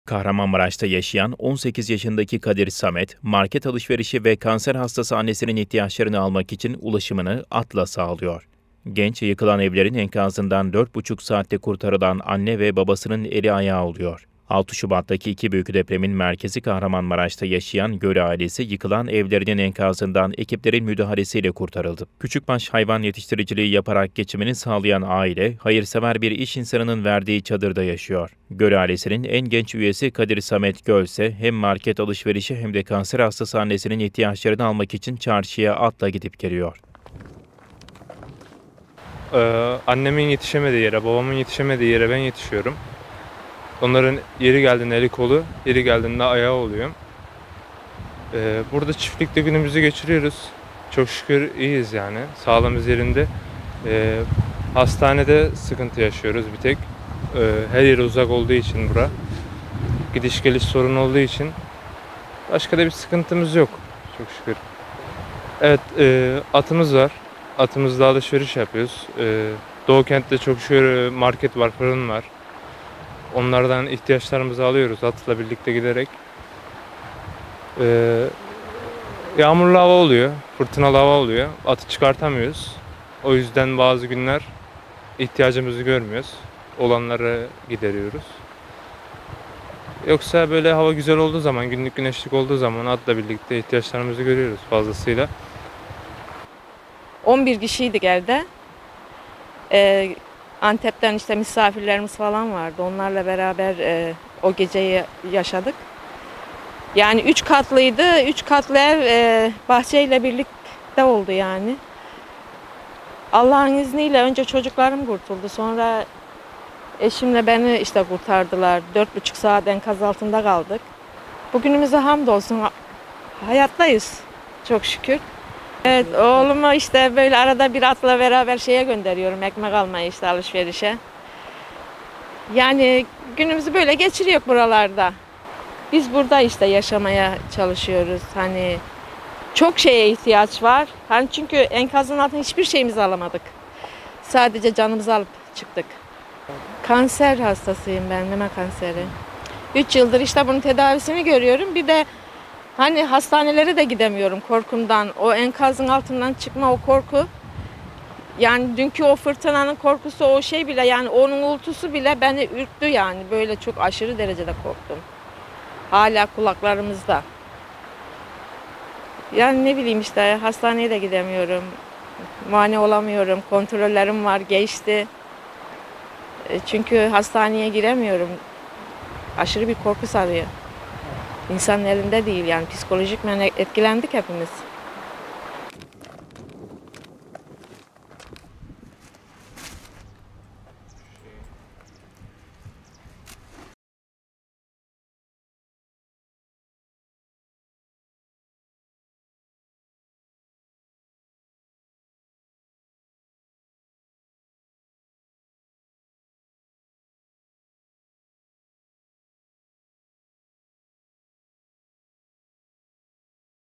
Depremzede Genç Market Alışverişine At İle Gidiyor
Kahramanmaraş'ta yaşayan 18 yaşındaki Kadir Samet, market alışverişi ve kanser hastası annesinin ihtiyaçlarını almak için ulaşımını at ile sağlıyor. Genç, yıkılan evlerinin enkazından 4,5 saatte kurtarılan anne ve babasının eli ayağı oluyor.

6 Şubat'taki iki büyük depremin merkezi Kahramanmaraş'ta yaşayan Göl ailesi, yıkılan evlerinin enkazından ekiplerin müdahalesiyle kurtarıldı. Geçimini küçükbaş hayvan yetiştiriciliği yaparak geçimini sağlayan aile, hayırsever bir iş insanının verdiği çadırda yaşıyor. Göl ailesinin en genç üyesi Kadir Samet Göl ise hem market alış verişi hem de kanser hastası annesinin ihtiyaçlarını almak için çarşıya at ile gidip geliyor.
18 yaşındaki Kadir Samet, İHA muhabirine yaptığı açıklamada, annesinin ve babasının eli ayağı olduğunu belirterek, çarşının uzak olmasından dolayı alışverişe atı ile gittiğini söyledi.

"Çok şükür iyiyiz sağlığımız yerinde"
Yaşadıkları bölgenin çarşı merkezine uzak olduğunu ifade eden Kadir Samet Göl, "Annemin babamın yetişemediği yere ben yetişiyorum. Onların yeri geldiğinde eli kolu, yeri geldiğinde de ayağı oluyorum. Burada günümüzü geçiyoruz çok şükür iyiyiz sağlığımız yerinde. Hastanede sıkıntı yaşıyoruz. Burası her yere uzak olduğu için. Gidiş geliş sorunu olduğu için. Başka da bir sorunumuz yok" dedi.

"At ile birlikte ihtiyaçlarımızı görüyoruz"
Göl, "Atımız var. Atımız ile alışveriş yapıyoruz. Doğu kentte çok şükür market ve fırın var. Atımla birlikte giderek ihtiyaçlarımızı alıyorum. Yağmurlu fırtınalı hava oluyor atı çıkartamıyoruz o yüzden bazı günler ihtiyacımızı görmüyoruz. Olanlarla idare ediyoruz. Böyle hava güneşli olduğu zaman da at ile birlikte ihtiyaçlarımızı görüyoruz" diye konuştu.

"11 kişiydik evde"
Eşiyle birlikte enkazda 4,5 saat kaldıklarını anlatan kanser hastası Ülkiye Göl ise "11 kişiydik evde. Gaziantep'ten misafirlerimiz vardı. Onlarla beraber o geceyi yaşadık. 3 katlıydı ev. Allah'ın izniyle önce çocuklarım kurtuldu. Sonrada eşimle beni kurtardılar. 4,5 saat enkaz altında kaldık. Hayattayız çok şükür" şeklinde konuştu.
3 yıldır meme kanseri tedavisi gördüğünü belirten anne Göl, "Oğlumu atla beraber ekmek almaya gönderiyorum alışverişe. Biz burada yaşamaya çalışıyoruz. Çok şeye ihtiyaç var. Çünkü enkazın altından hiçbir şeyimizi alamadık. Sadece canımızı alıp çıktık. Meme kanseri hastasıyım. 3 yıldır bunun tedavisini görüyorum. Hastaneye de gidemiyorum korkumdan. Fırtına bile beni korkuttu. Kontrollerim var geçti. Çünkü hastaneye giremiyorum. Aşırı bir korku sarıyor. İnsanın elinde değil psikolojik olarak etkilendik" dedi.